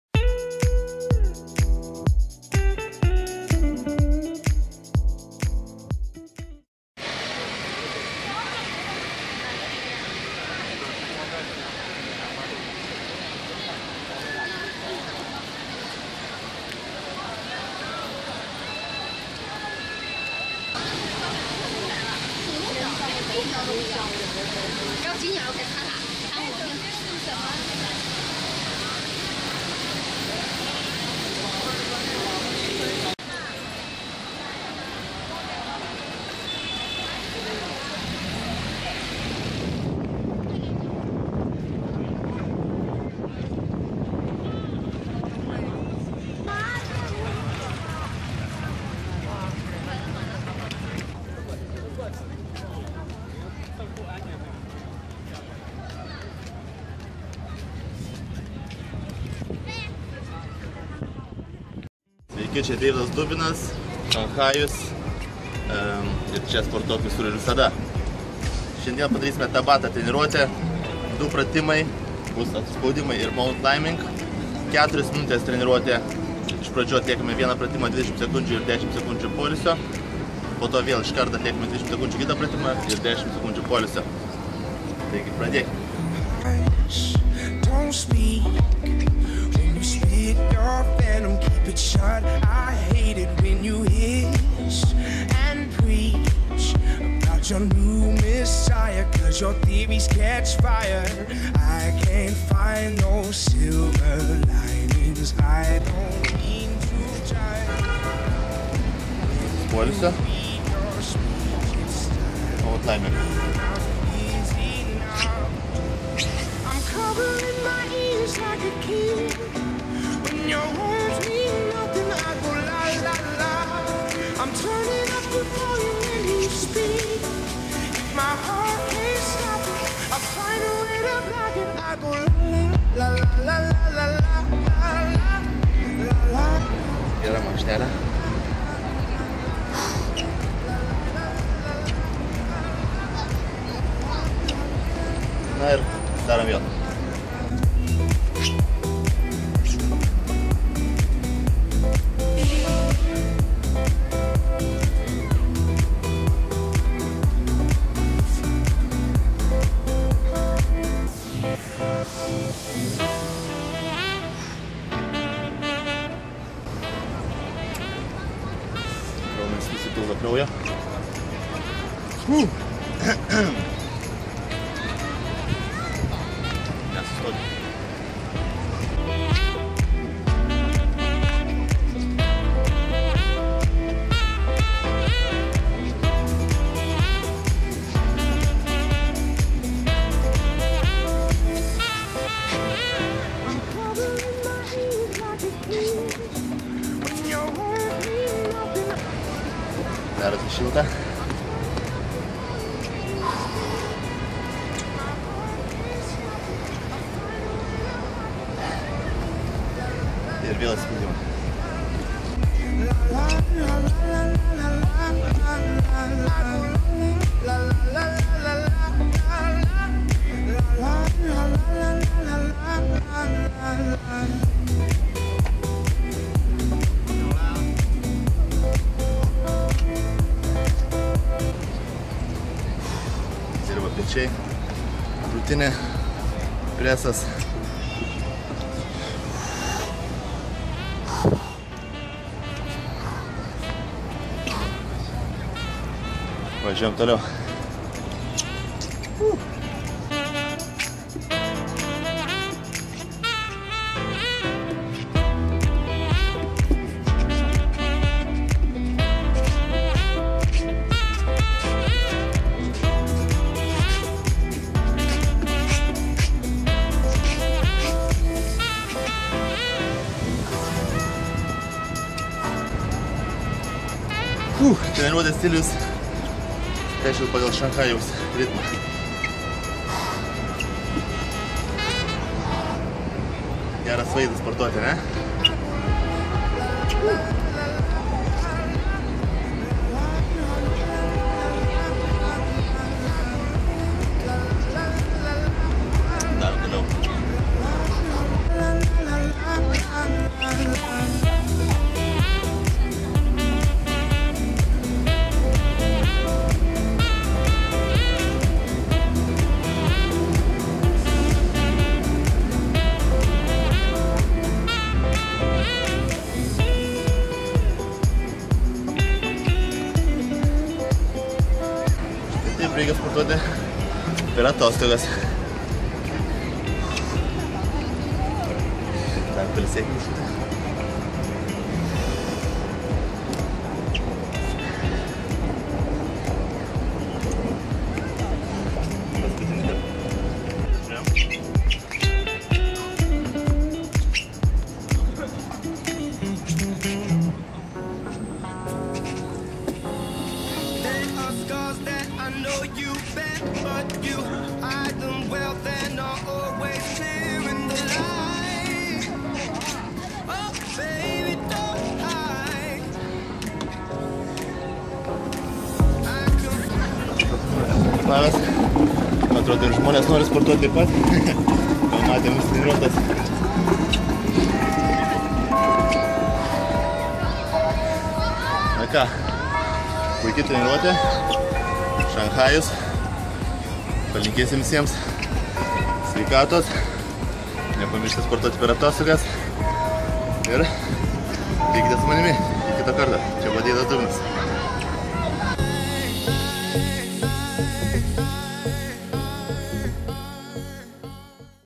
Deividas Dubinas Training Edition: 4 min Tabata Workout
Atostogų metu nesustojame...Judesys Šanhajuje...Tabata workout baby! Tabata – tai intensyvi aerobika, tinkanti faktiškai visiems. Tabatos treniruotė - tai intervalais atliekami pratimai stengiantis atlikti kuo teisingiau ir kuo daugiau kartų vienos serijos metu. Visa treniruotė trunka ~4 min:

Viena serija – 20 sekundžių
Poilsis – 10 sekundžių
Viso 8 serijos